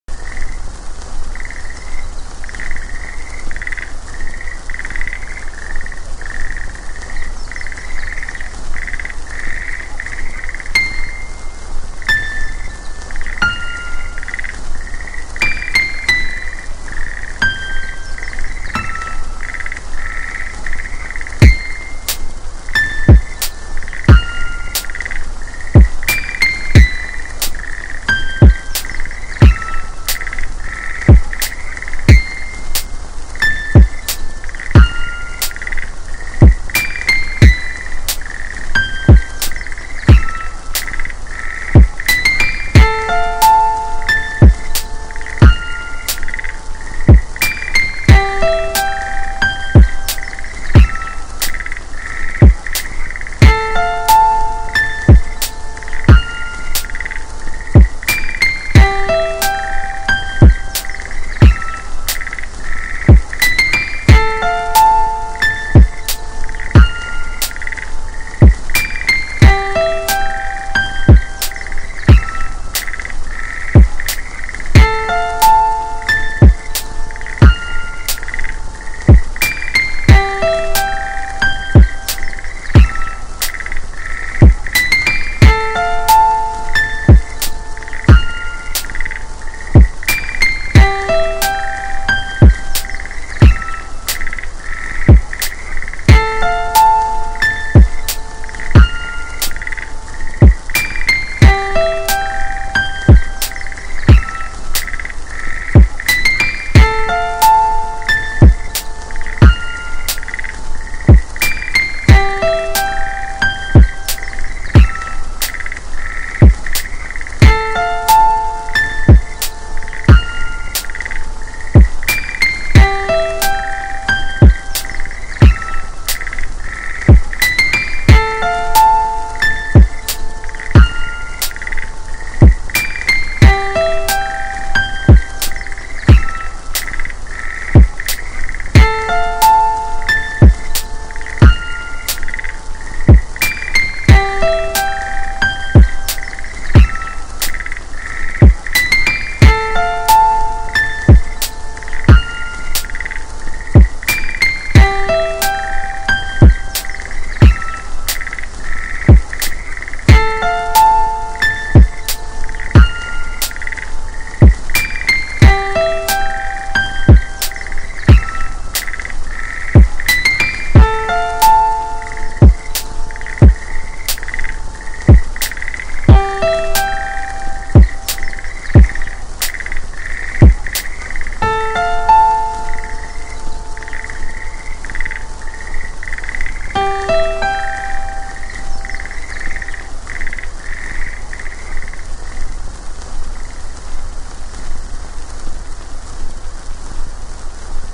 lofi hip hop for rainy day/chill night
This is Naru who act as a lifestyle explorer.
I made the song when I missed to move big island of Hawaii.
This song fit in rainy day and chilling night.

こんにちは！生き方冒険家のナルです🚚✨

2018年にハワイ移住できず帰ってきた際に作ったlofi hip hopのデータがあったのでアップロードしました！
雨の昼、落ち着いた夜に会う曲だと思います。
作業用BGMとして聞いてみてください！